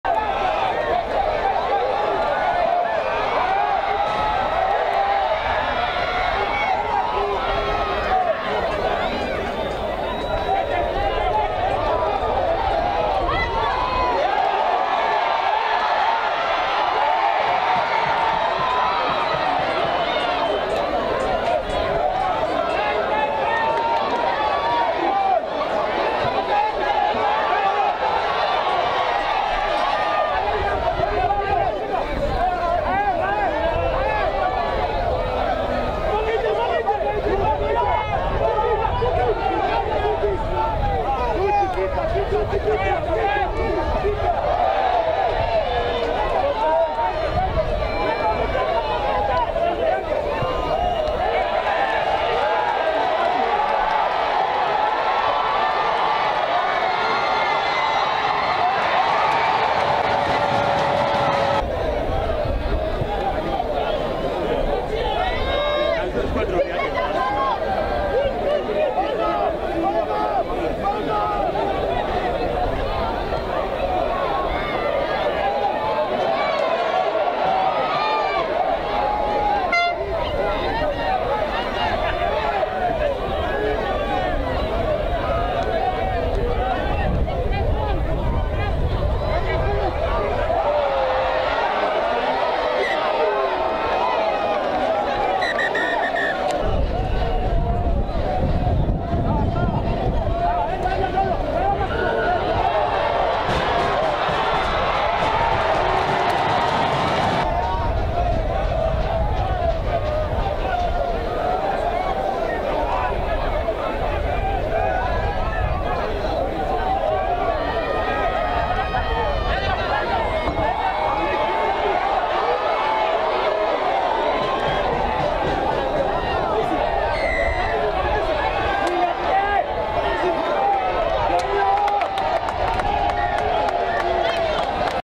Strathmore Leos vs Menengai Oilers National Sevens Circuit 2022 Prinsloo7s Cup Final Rugby Highlight
Played on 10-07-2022 in Nakuru Athletics Club